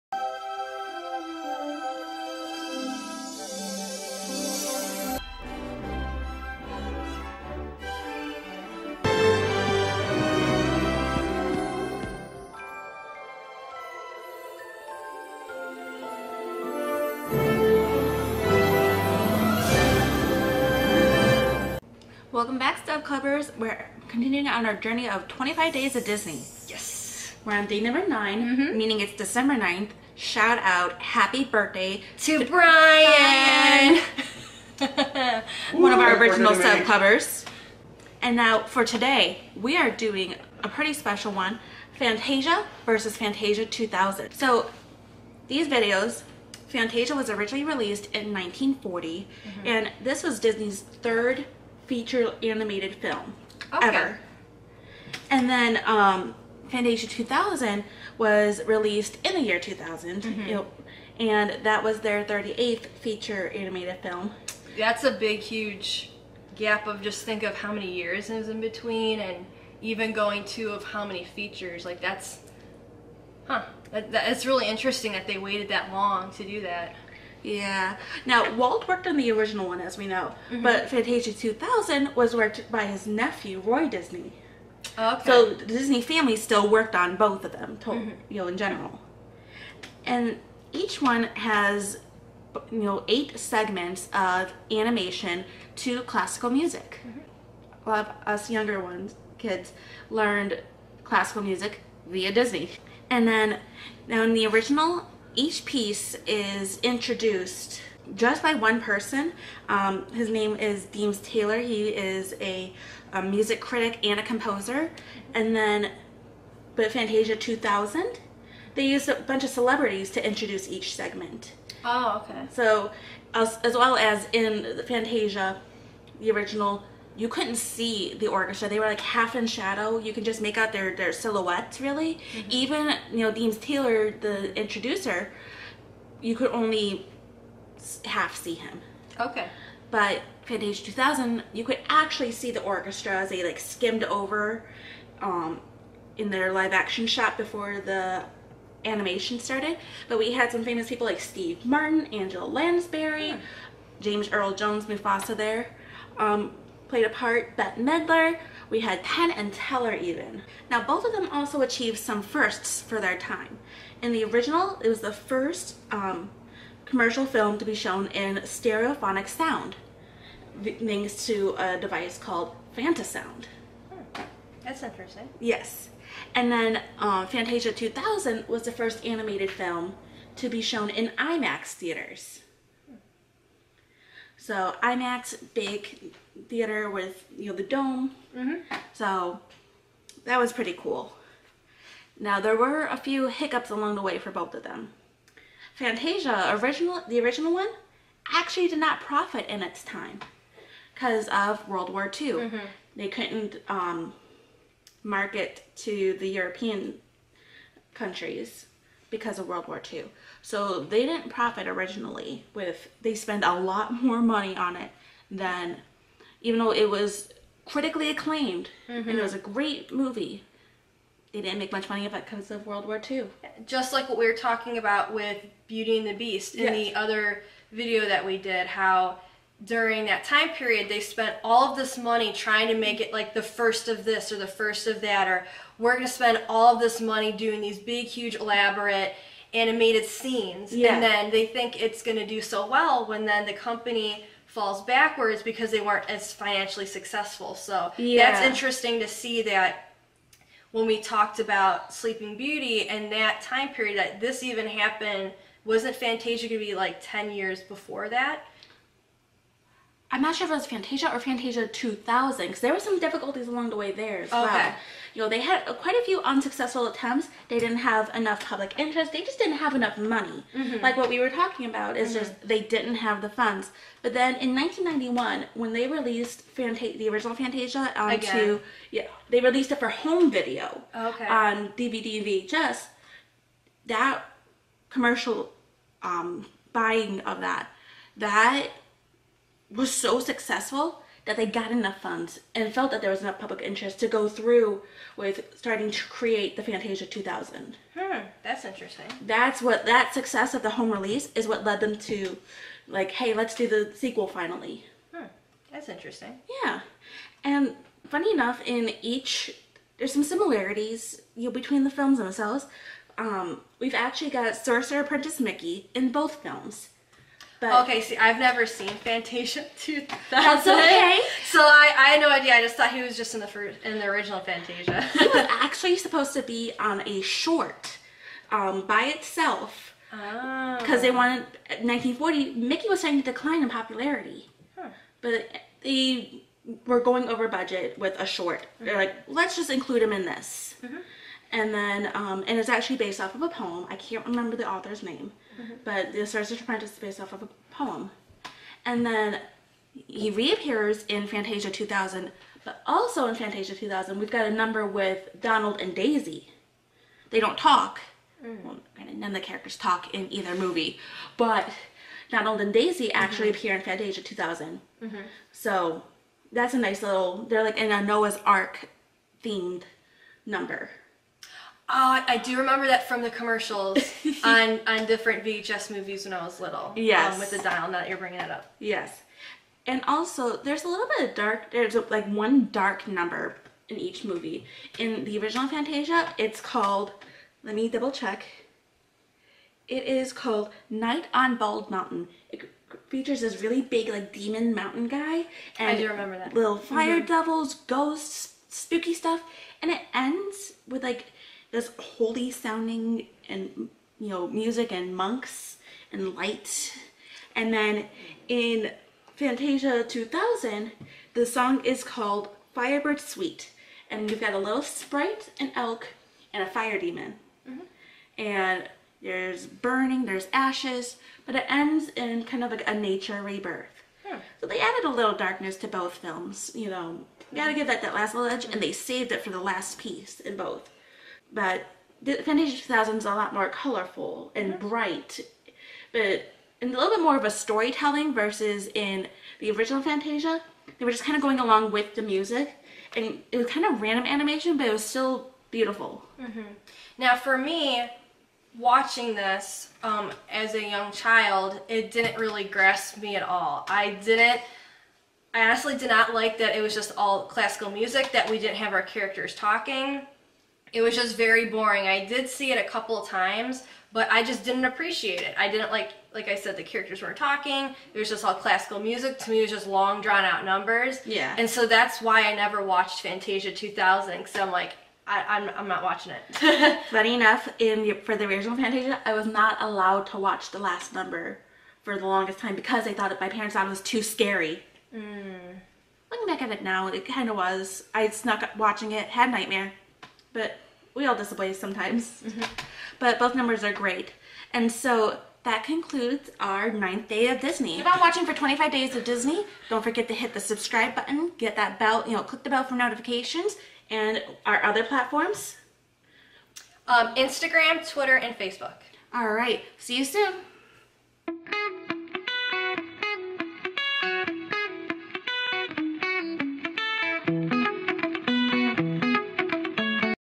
Fantasia vs Fantasia 2000 | 25 Days of Disney Day 9
Stub Clubbers, welcome to 25 Days of Disney, Day 9 , where we talk about Fantasia vs Fantasia 2000.